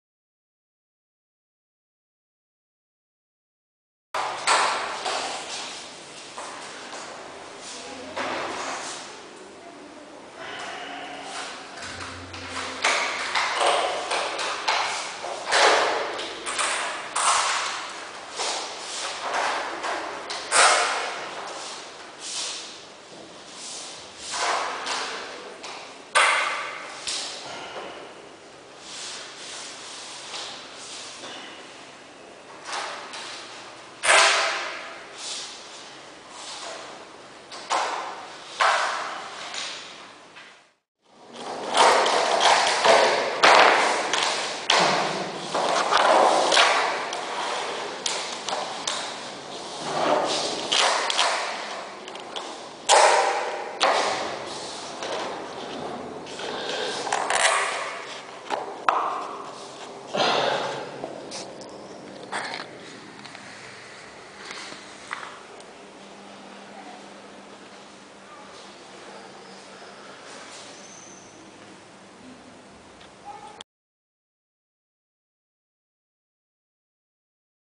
Монтаж старомодного пластикового плинтуса
Нашел давнее видео где такой плинтус когда-то монтировали. Что характерно недавно такой же плинтус мне дали для монтажа. У людей лежал этот плинтус в гараже несколько лет и тут решились на ремонт. На те времена это был бюджетный плинтус, который крепился на специальные кляймера. Самым худшим было в этой установке когда вдобавок нужно было спрятать под плинтусом кабеля. Все плевались от такого вида крепления, но он долгое время существовал. Качество не ахти, но те кто помнит - сразу узнает